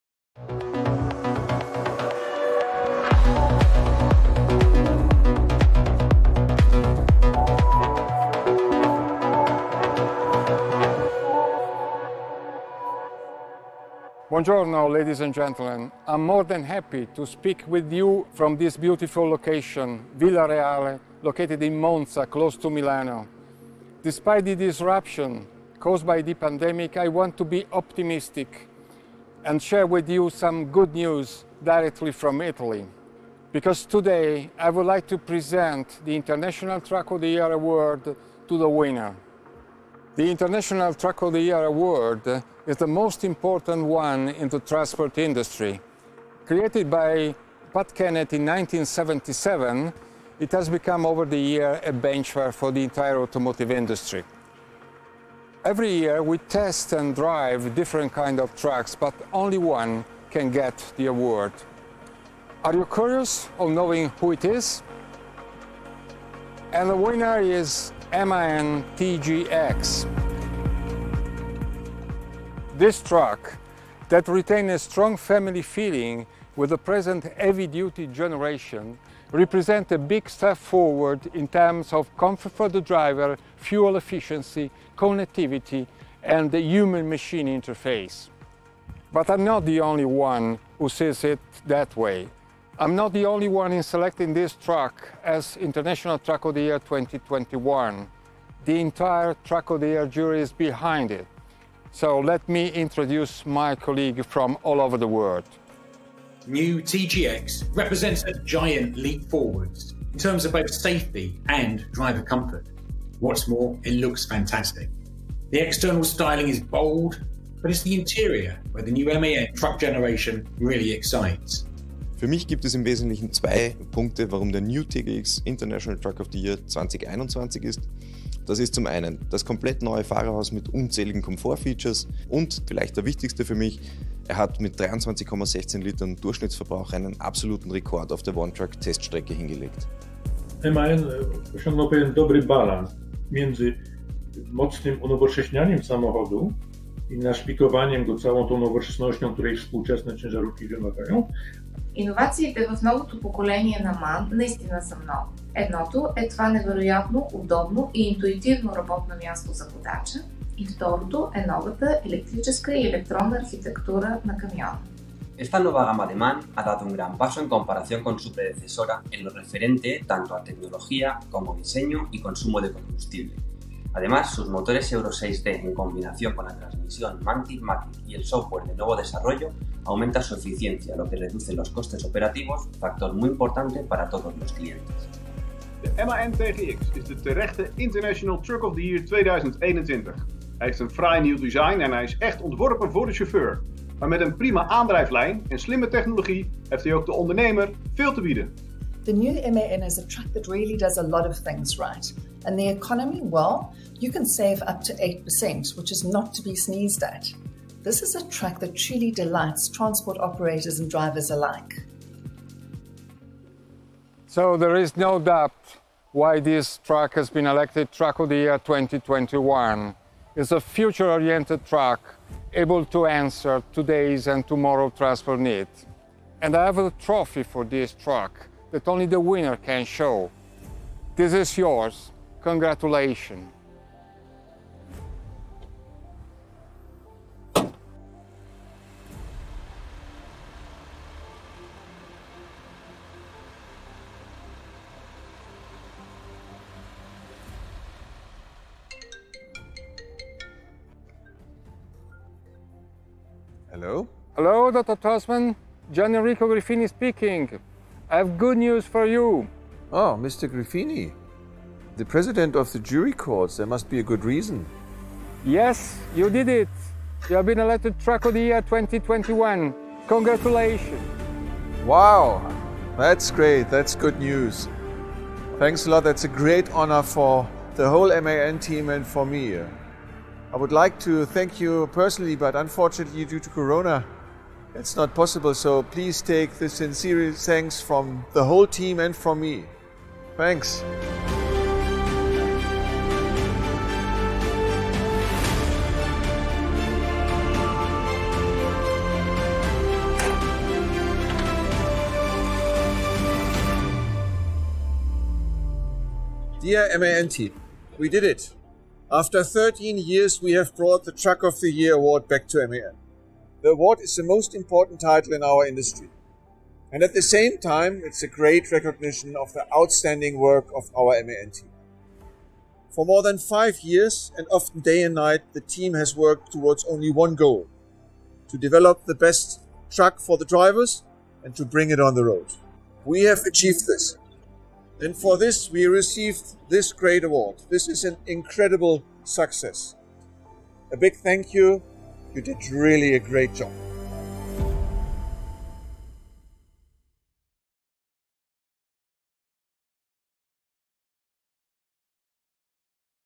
Az új MAN TGX -- International Truck of the Year 2021! (MAGYAR FELIRATTAL)
Nagy megtiszteltetés, hogy 2021-ben az új MAN TGX elnyerte a rangos "International Truck of the Year 2021" címet.